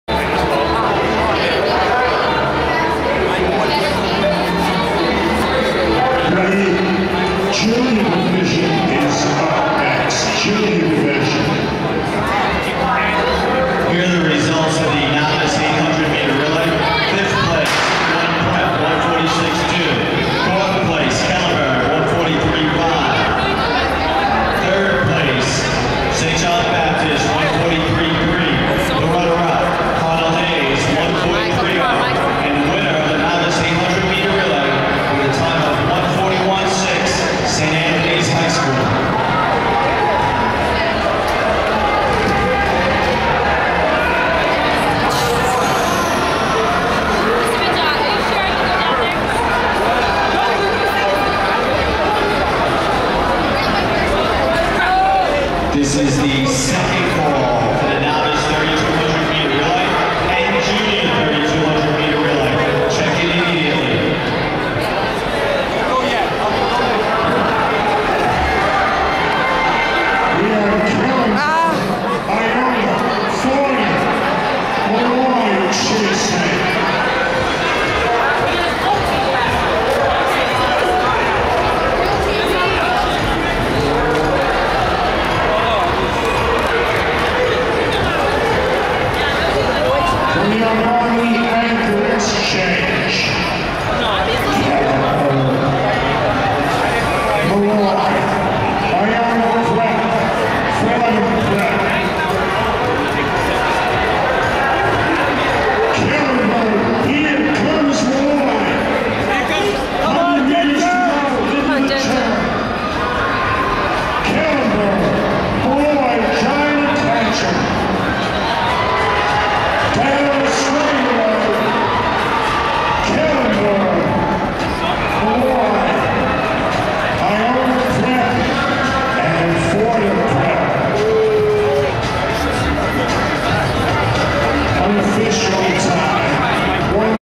CHSAA Weight Class Championship 2009 - Junior Weight (under 125lbs) 4 x 200
This was the 4x200 junior weight class.   Iona Prep starts in 2nd place but drops to 3rd after a couple of slow passes.   They ended in 3rd place.